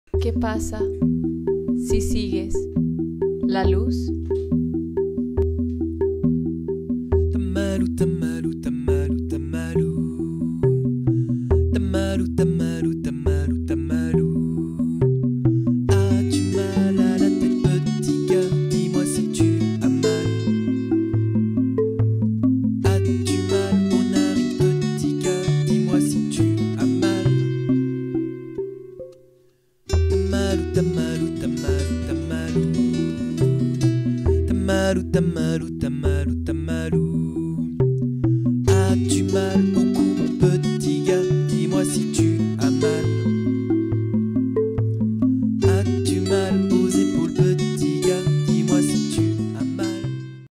Pregunta X para Respuestas X: La luz
Nuestro equipo de versión beta se preguntó por la luz y a partir de ahí, las múltiples respuestas surgieron.  Una animación maravillosa para hablar de eso que alumbra las cosas y de algún modo les da vida. ¿qué pasa si sigues la luz?